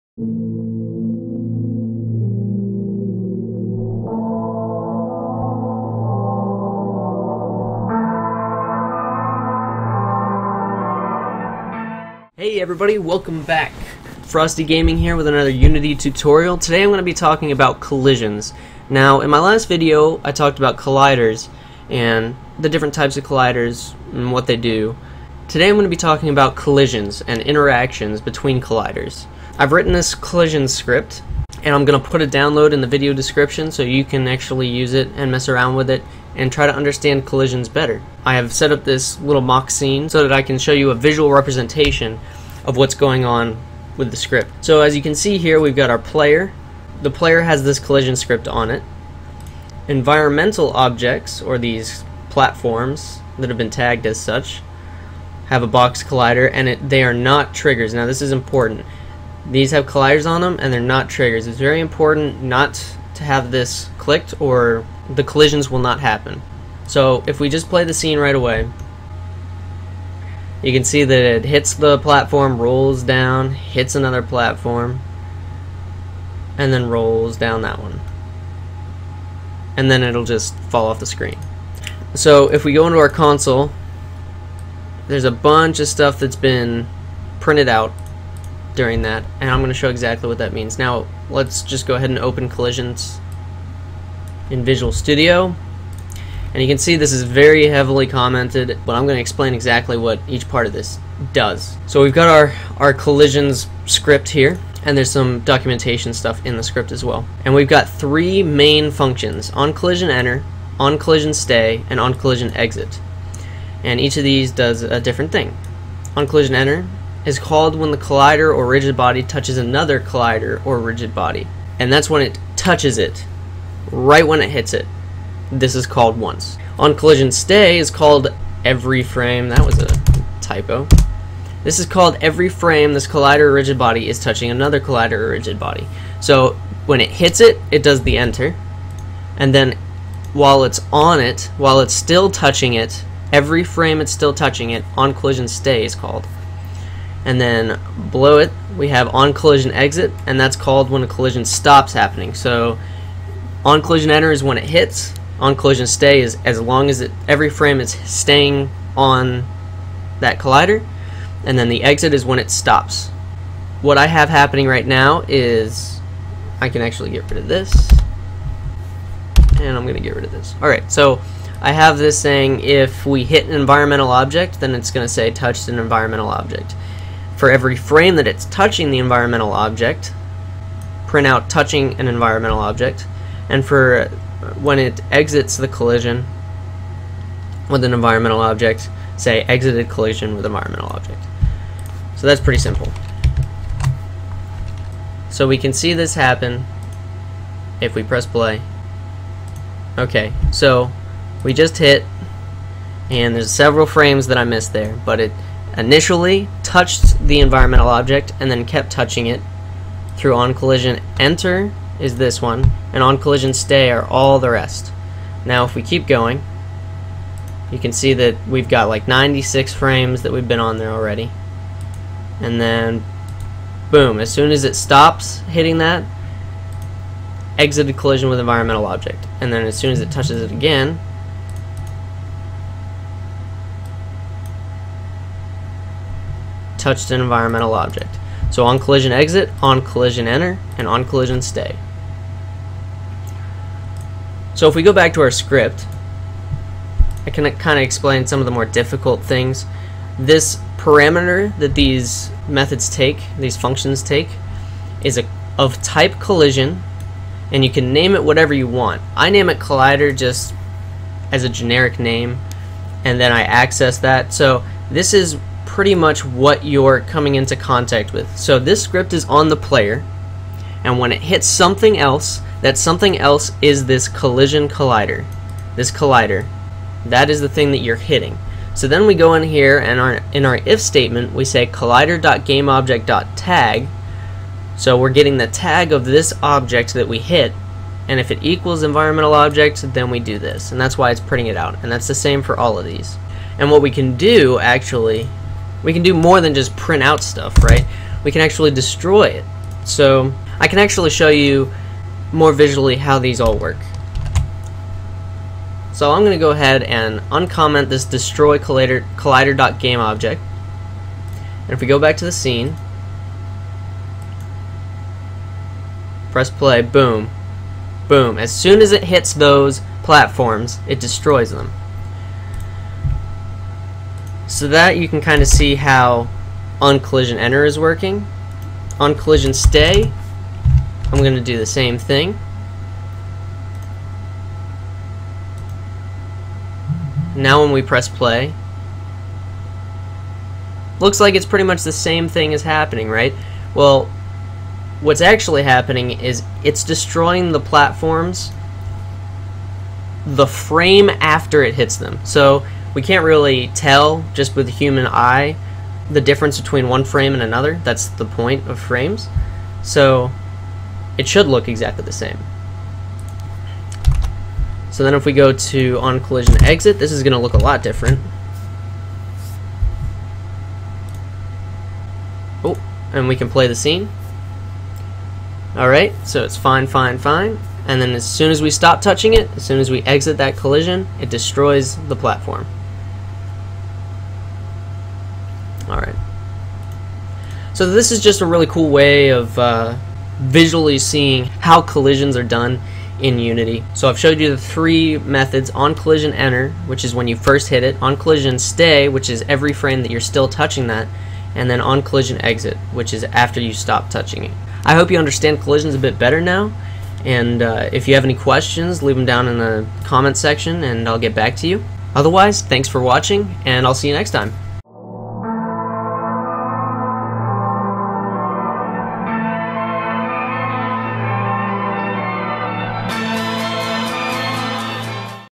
Beginner Unity Basics #18 - Collisions
Welcome back!  Today I'll be talking about Collisions, specifically the different methods we can use to make things happen during collisions:

-OnCollisionEnter()
-OnCollisionStay()
-OnCollisionExit()

These three functions can be used to debug or destroy or really anthing you really want to do in relation to collisions.  I show you a simple script that I'll be making available soon which helps to explain these three methods.  For more information you can check out these links or look at the Unity Scripting API by simply searching in google the name of these methods.

You can also check out my facebook page and twitter for information on my videos and game development work: